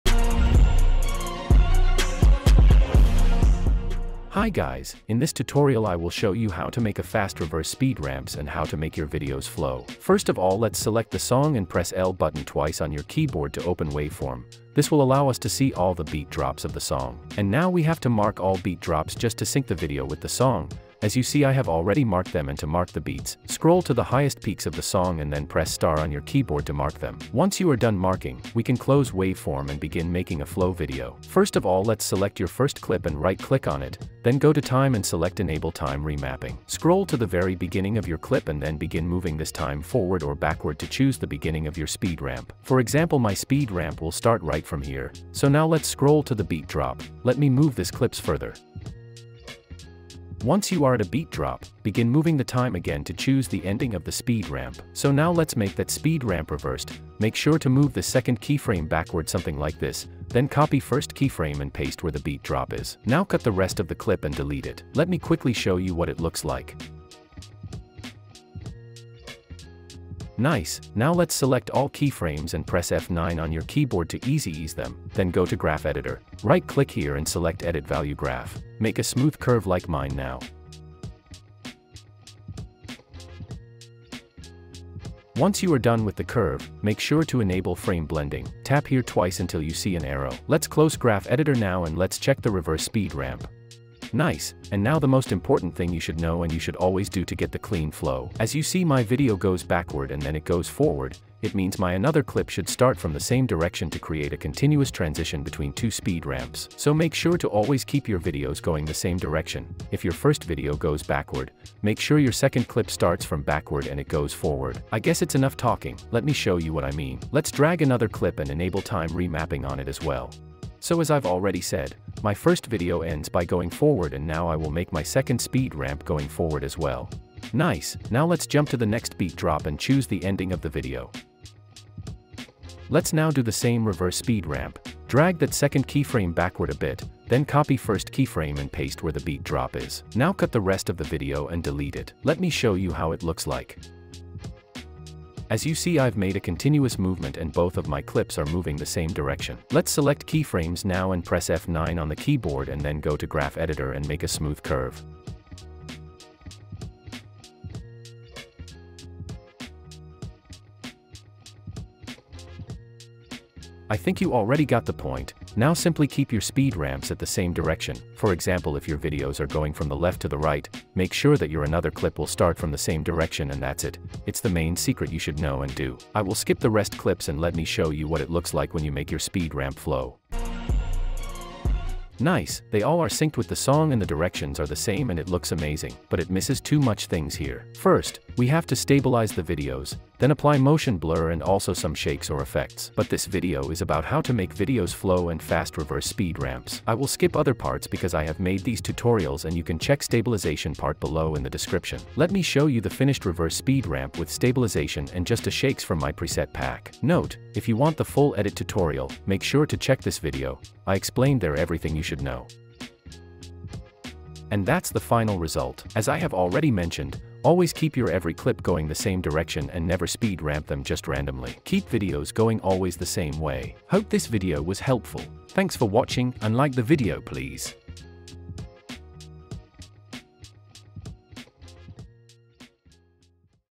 Fast Reverse Speed Ramp in After Effects (Make Your Videos Flow)
Learn how to create a fast reverse speed ramp in After Effects to make your videos flow smoothly and look professional. This tutorial covers speed ramping techniques, reverse video effects, and dynamic transitions that will enhance your editing skills and take your videos to the next level.

CPU: i7-14700K
GPU: RTX 4060 Ti 16GB
RAM: Kingston FURY Beast 32GB